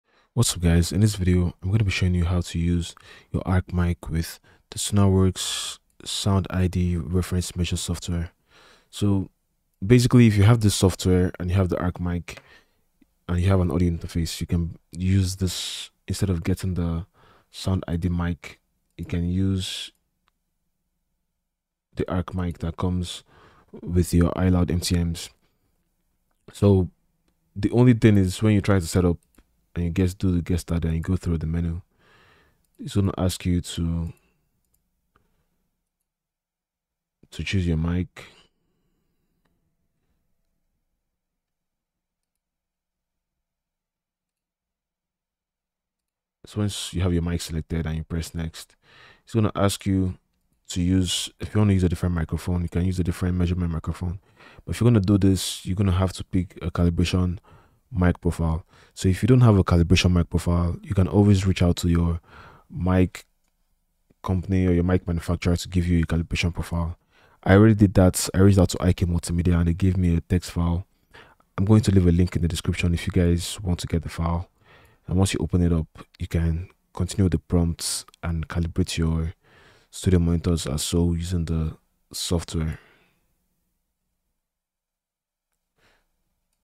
Use IK Multimedia ARC mic with Sonarworks SoundID Reference Measure Software to Measure your Room.
how to use a custom mic with SoundID reference Measure software

How to use IK Multimedia ARC mic with Sonarworks SoundID Reference Measure Software to Measure your Room.